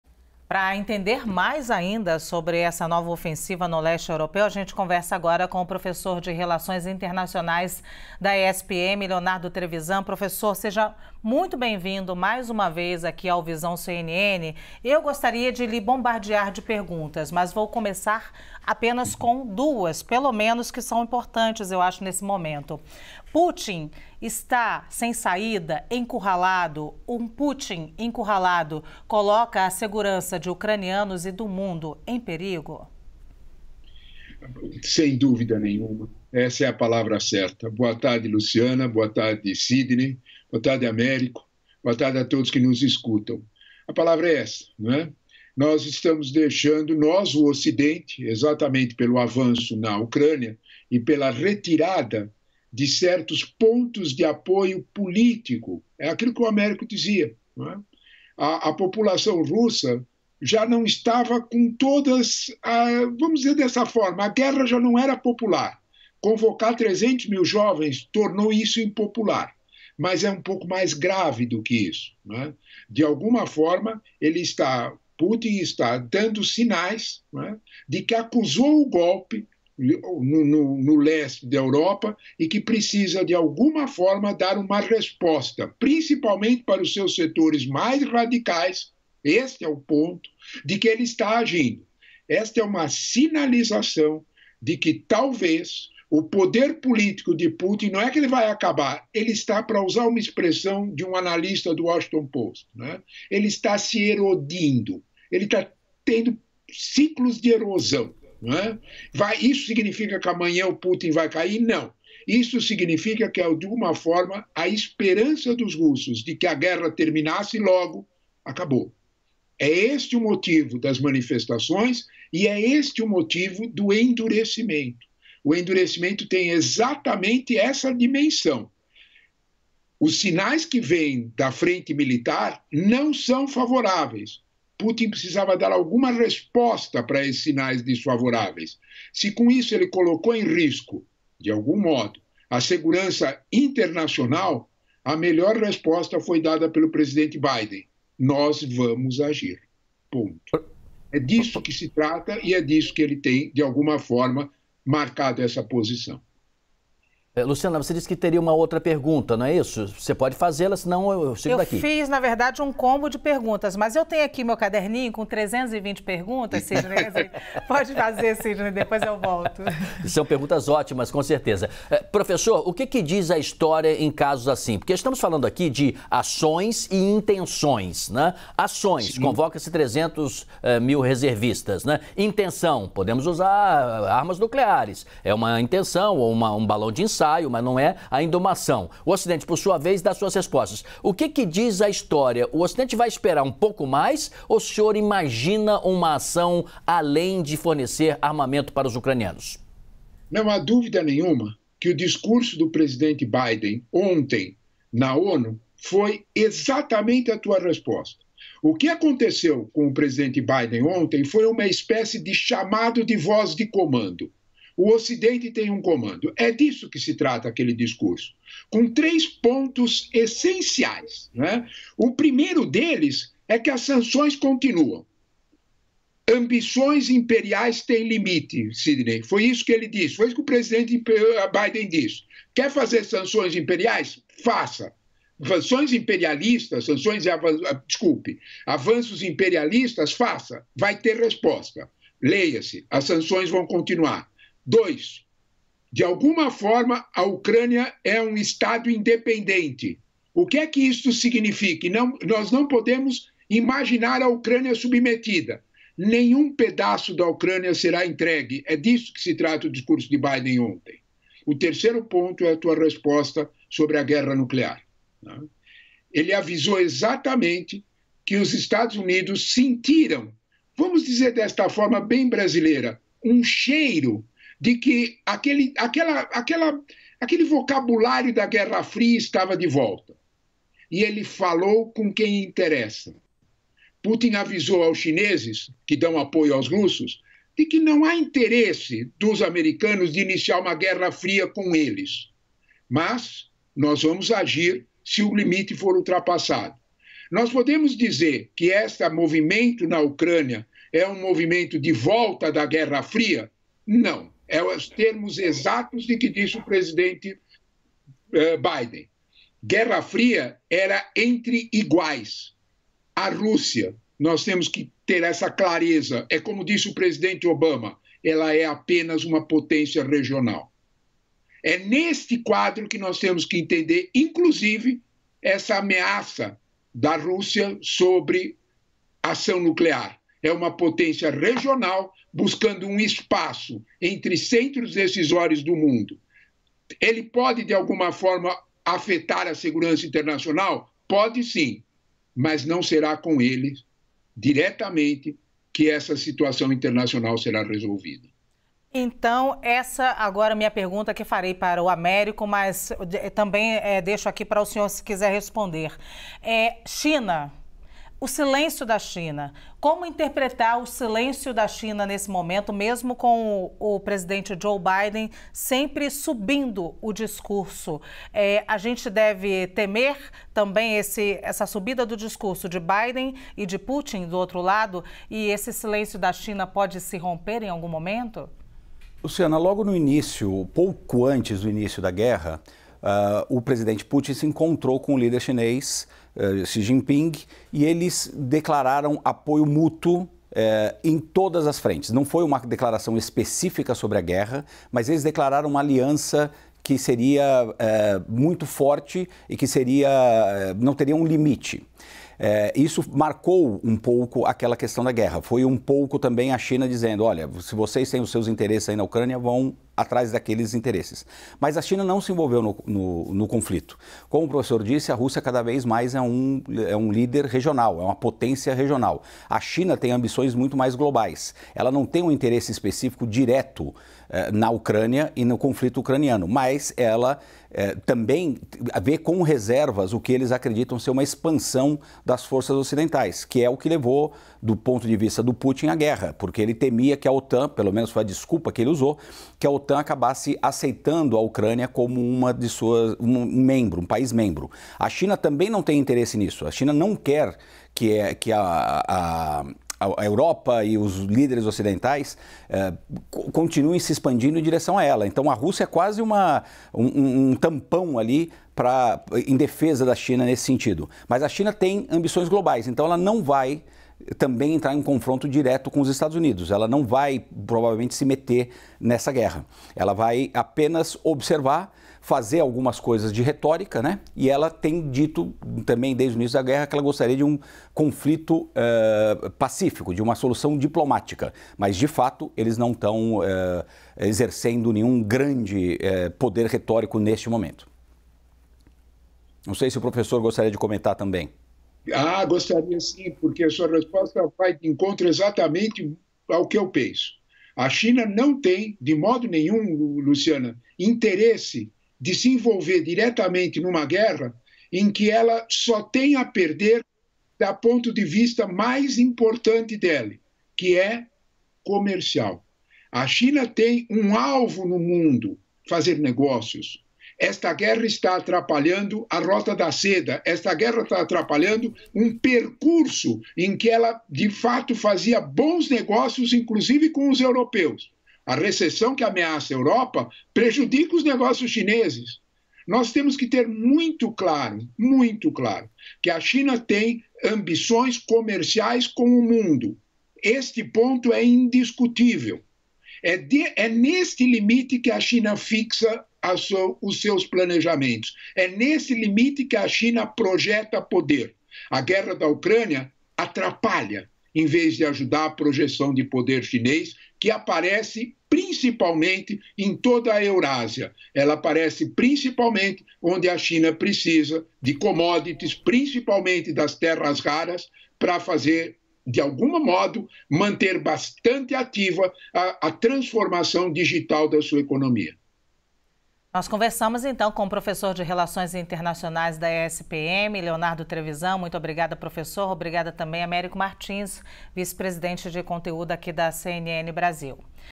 Ameaças de Putin são acenos aos seus setores mais radicais, diz professor | VISÃO CNN
Em entrevista à CNN nesta quinta-feira (22), o professor de Relações Internacionais Leonardo Trevisan, da Escola Superior de Propaganda e Marketing (ESPM), avaliou que as ameaças nucleares do presidente da Rússia, Vladimir Putin, contra a Ucrânia são um aceno a seus apoiadores mais radicais.